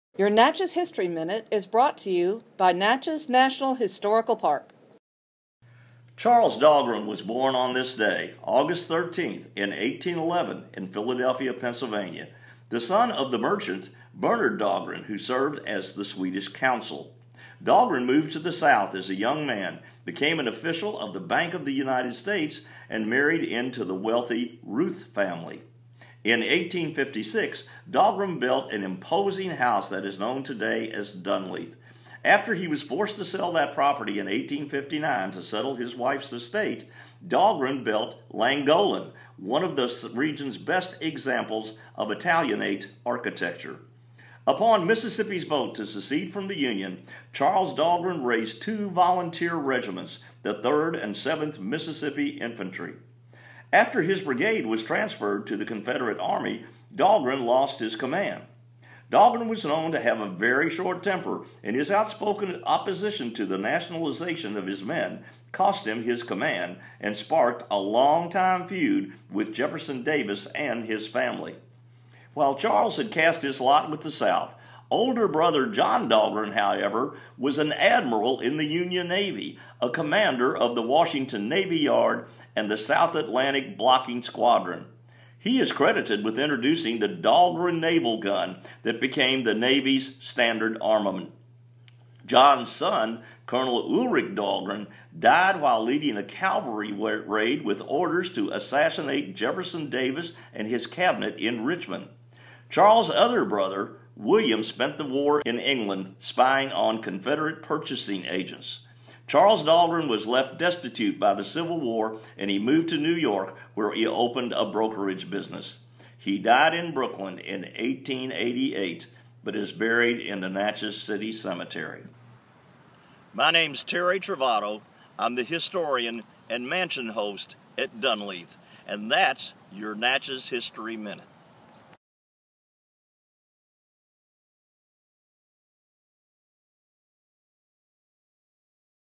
Aug 13 Brother vs Brother
Terry Trovato presents a Natchez History Minute about Charles Dahlgren, who was born on this day, August 13 in 1811.  Originally from Philadelphia, Pennsylvania, Dahlgren raised two regiments for the Confederacy but his brother, John became an admiral in the US Navy and his nephew died while conducting a raid to assassinate Jefferson Davis.